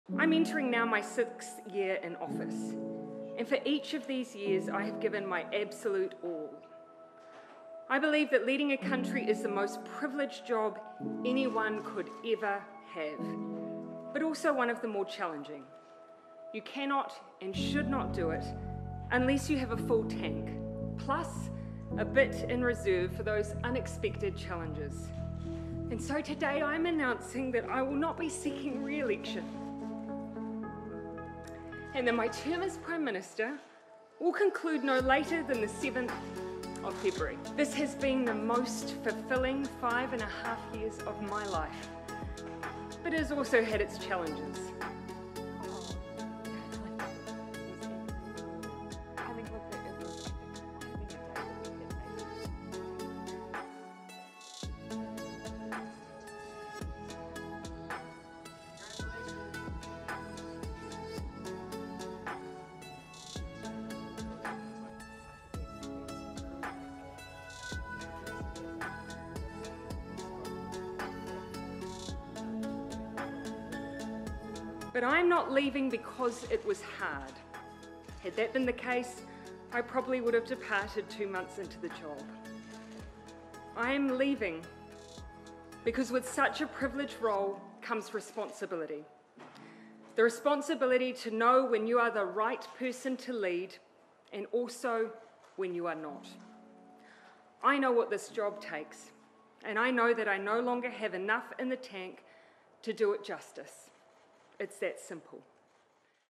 Jacinda Ardern announces shock resignation as New Zealand Prime Minister
Jacinda Ardern has announced her resignation as Prime Minister of New Zealand as she said she "no longer has enough in the tank" to lead. The premier will not contest this year's election and will step down on February 7.

In an emotional speech at the New Zealand Labour Party caucus on Thursday, Ardern said: "I know what this job takes. And I know that I no longer have enough in the tank to do it justice. It’s that simple I am human, politicians are human. We give all that we can for as long as we can. And then it’s time. And for me, it’s time."